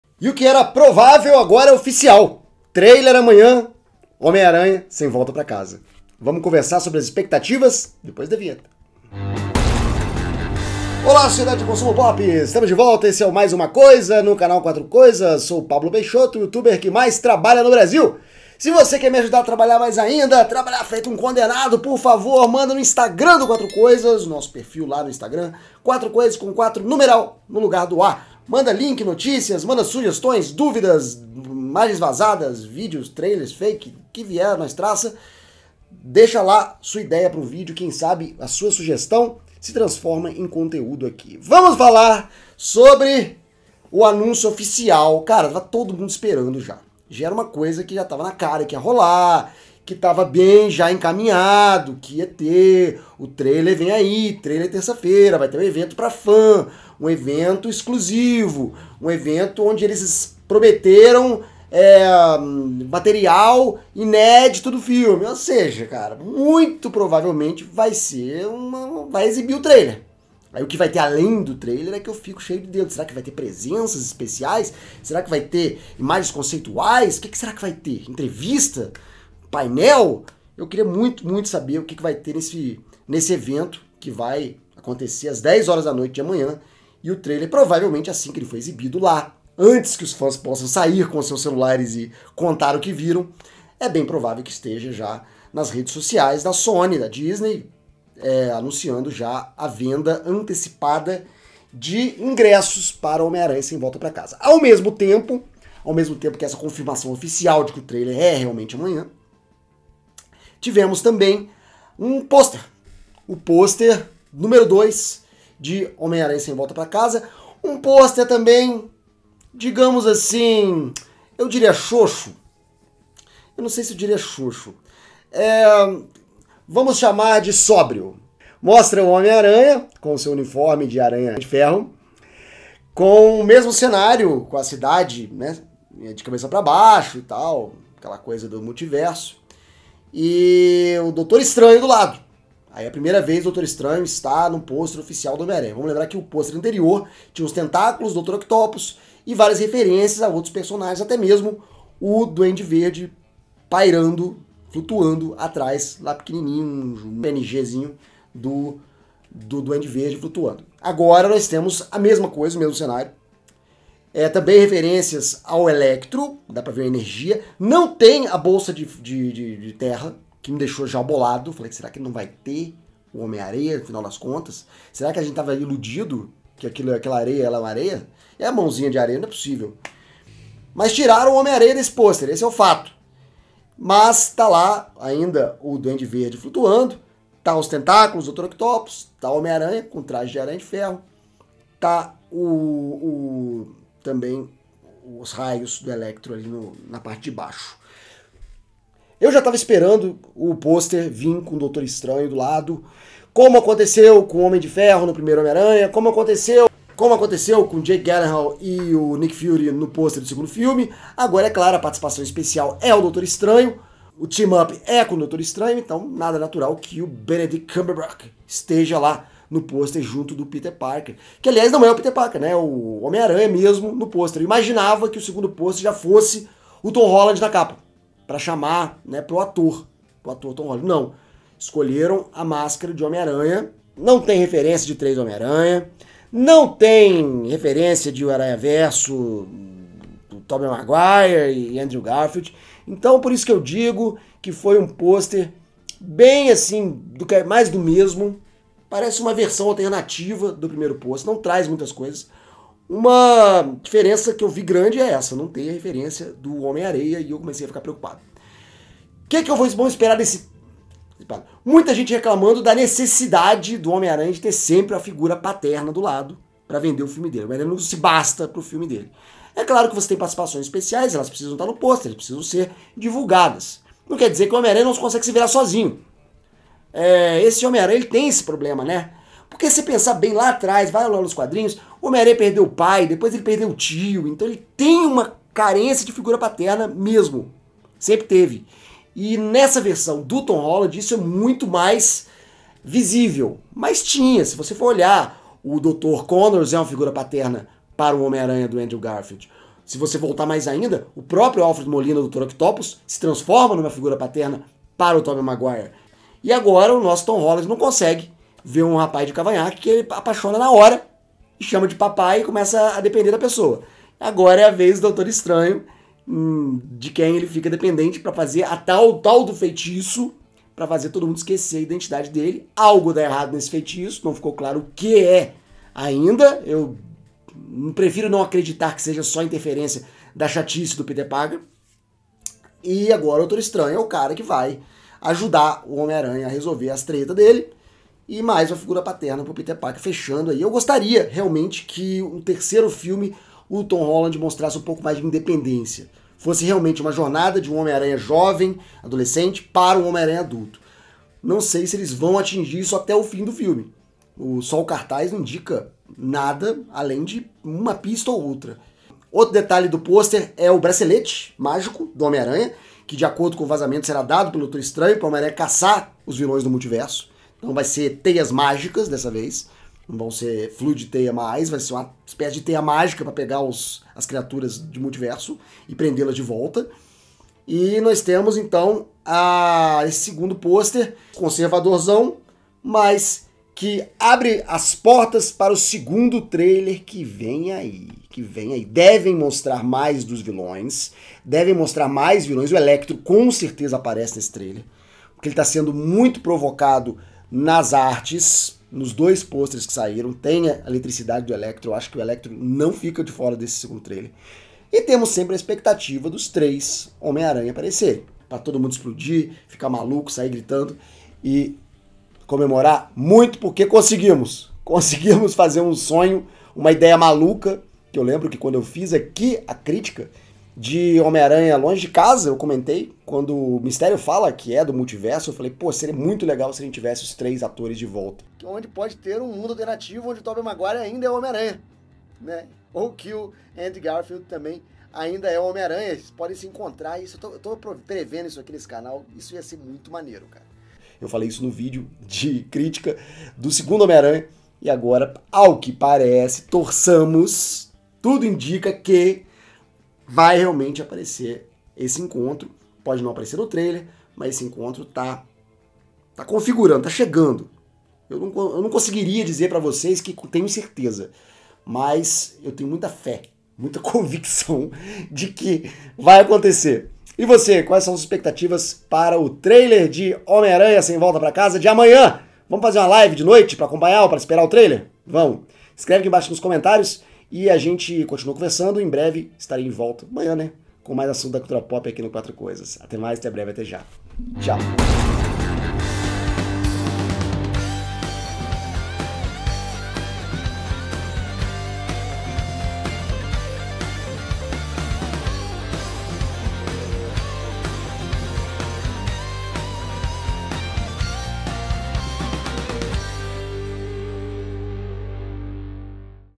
EU ESTOU SURTANDO! TRAILER DE HOMEM ARANHA SEM VOLTA PRA CASA SAI AMANHÃ EM EVENTO EXCLUSIVO.
Pablo Peixoto do Qu4tro Coisas fala sobre as expectativas para o lançamento do segundo e último trailer do filme Homem-Aranha Sem Volta pra Casa.